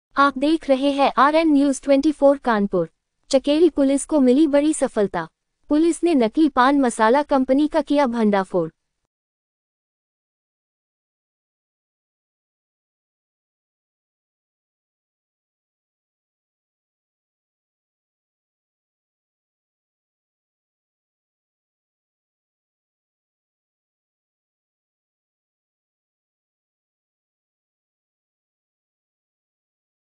kanpur चकेरी पुलिस को मिली बड़ी सफलता
कानपुर

चकेरी पुलिस को मिली बड़ी सफलता

पुलिस ने नकली पान मसाला कंपनी का किया भंडाफोड़